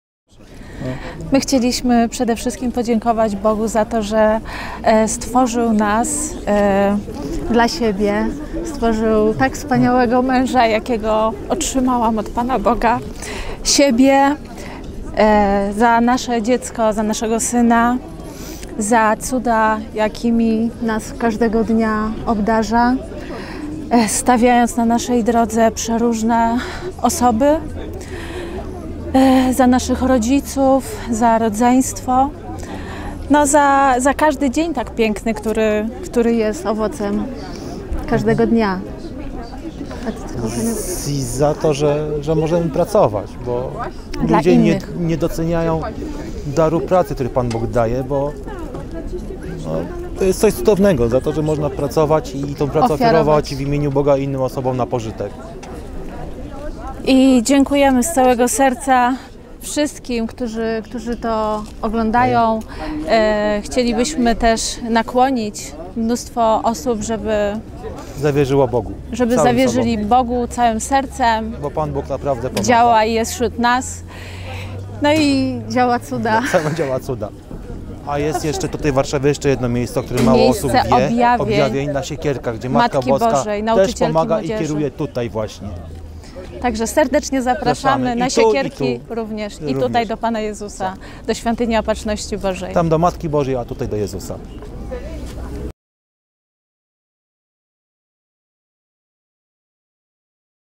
Świadectwo Barbary i Ernesta Rowickich z Warszawy - 30.03.2019 r.
centrumopatrznosci.pl
Świątynia Opatrzności Bożej, Msza św. dziękczynna za dar życia - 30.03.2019 r.